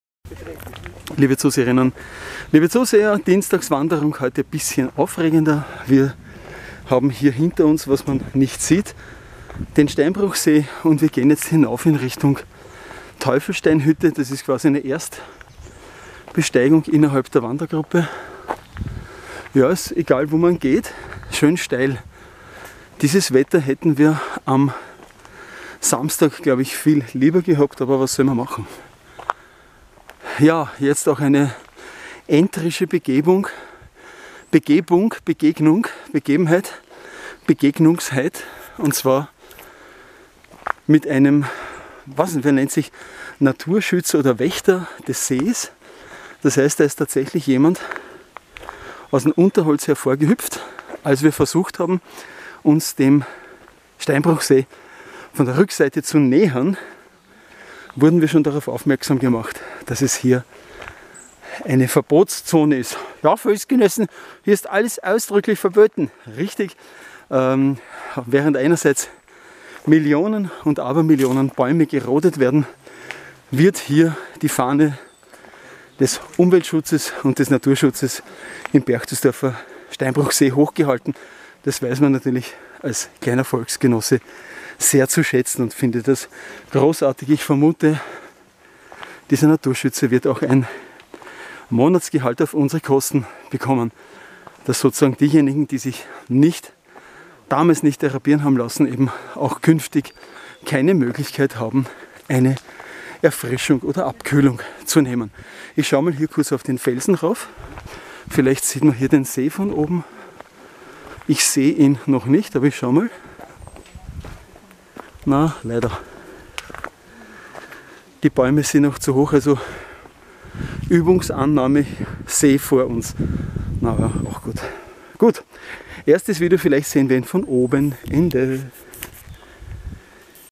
1/3 - Wanderung auf die TEUFELSTEINHÜTTE.
Das Wetter war heute ideal, sonnig doch nicht zu heiß. Eine gelungene Wanderung auf jeden Fall - wenn auch mit einigen steilen Passagen.